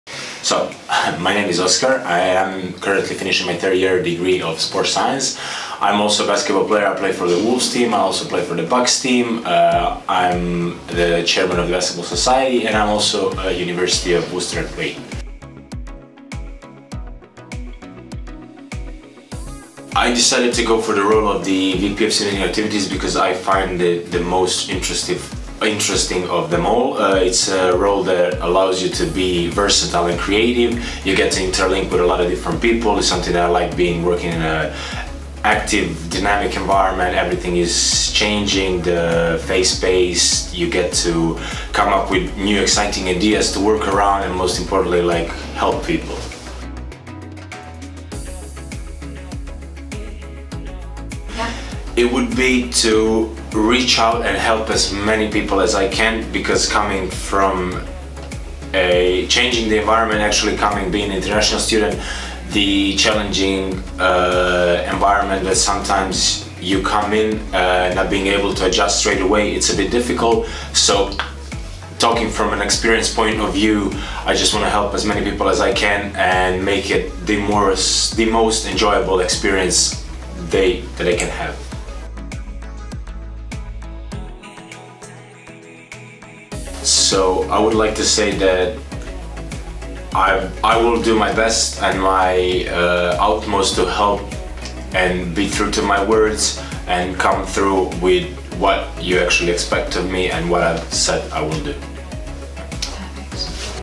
Oskar Amancic | Candidate for VP Student Activities
Meet Oskar, candidate for VP Student Activities at the Students' Union!

You can view the full list of candidates and their manifestos

Voting is open from Tuesday 12th March (2.15.pm) to Friday 15th March (2.15.pm).

Alternatively, you can vote at one of our polling stations in St John's Main Reception or Charles Hastings throughout the duration of the voting period.